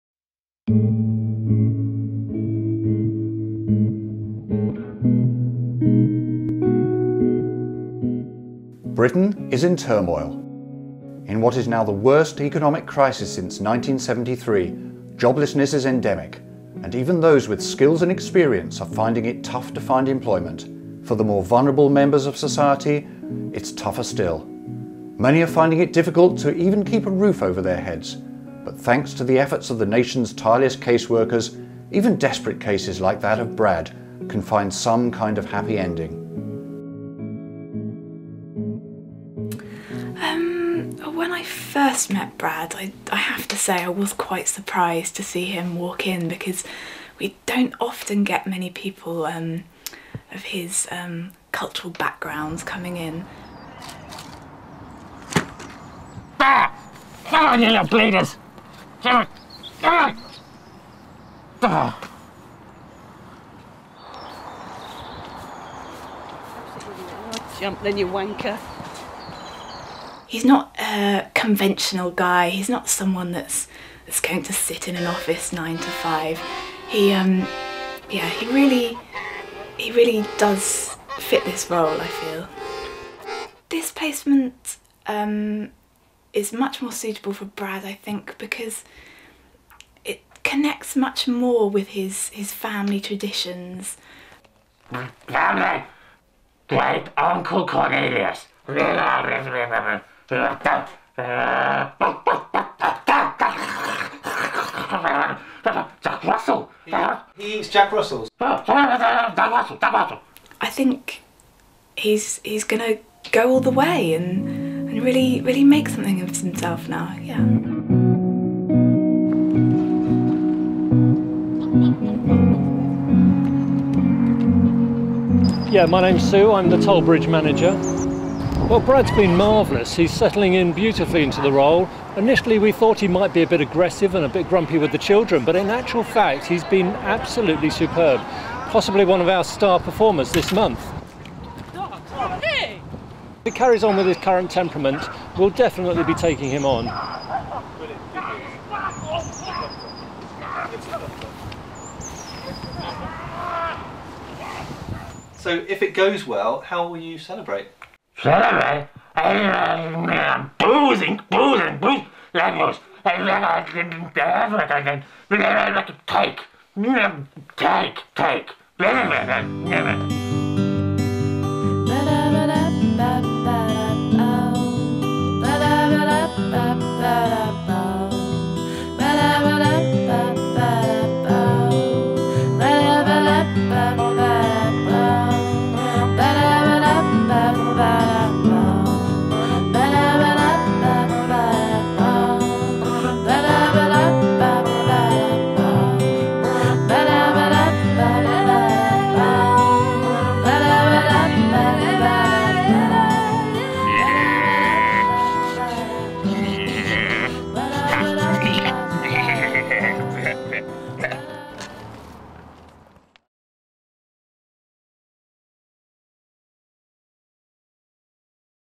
Toll Booth
Short film made on the Shooting Video course Spring 2012 about a toll booth and its new keeper.